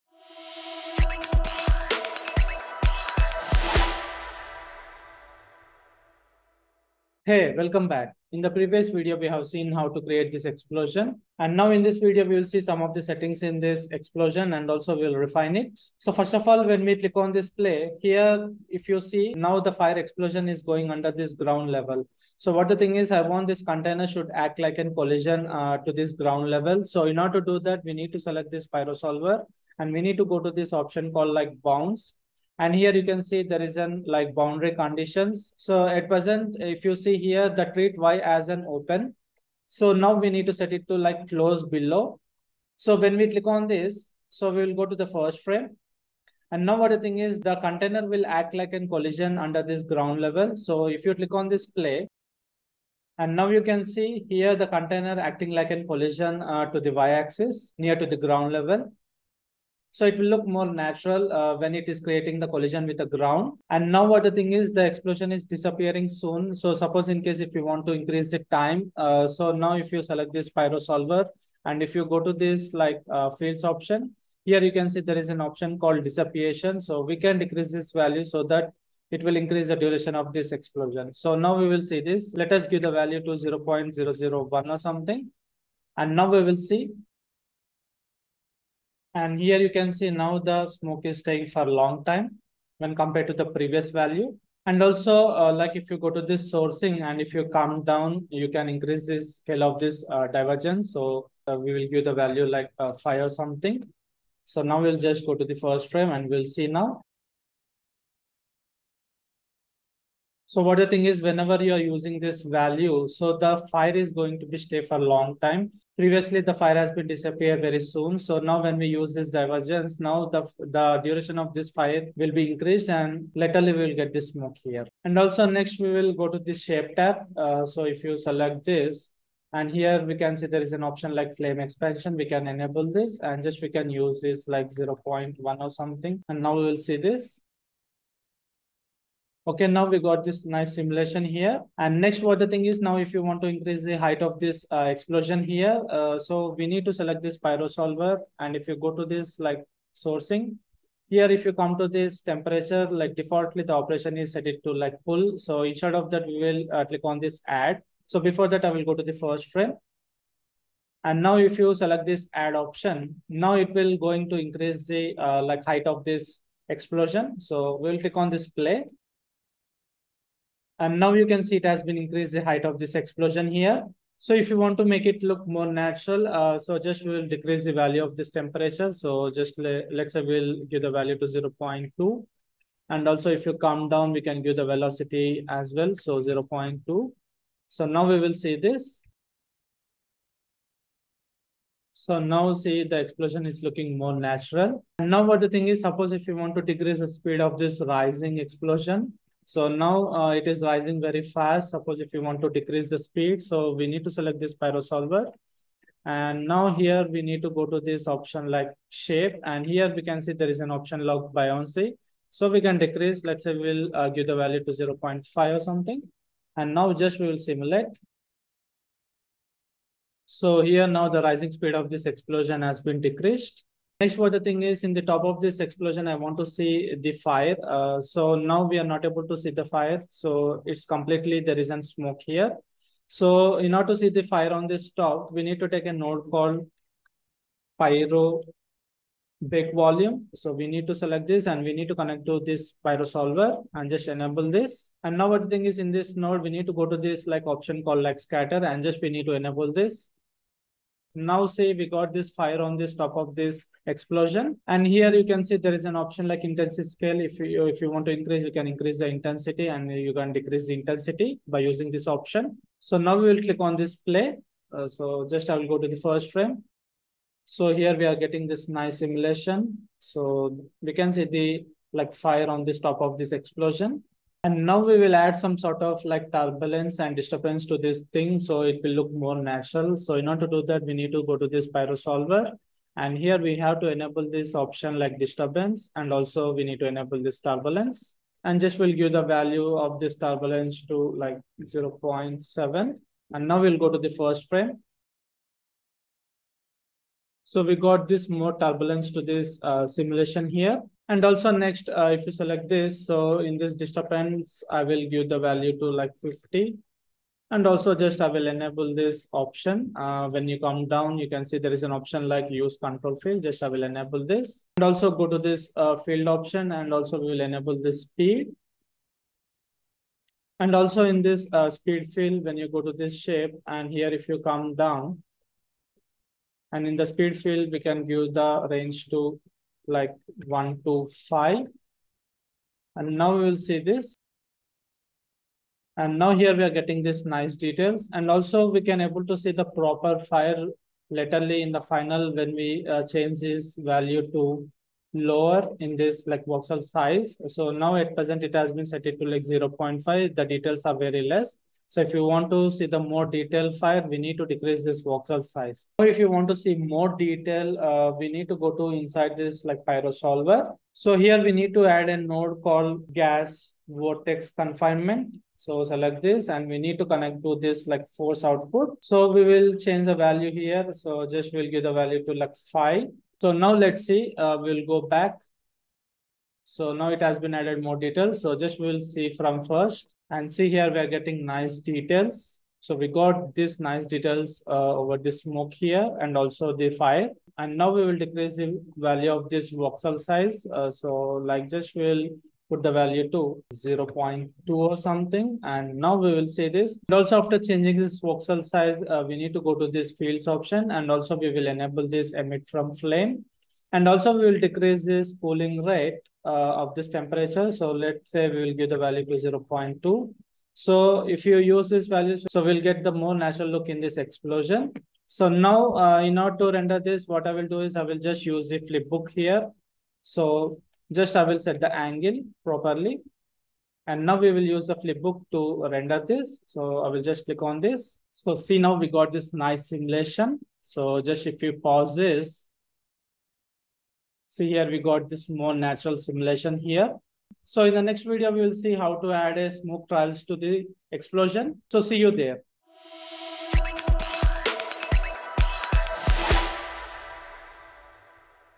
Taking Your Fire Explosions in Houdini Fx to the Next Level: Part 2
In this video we will see some adjustments in the simulations and will refine the explosion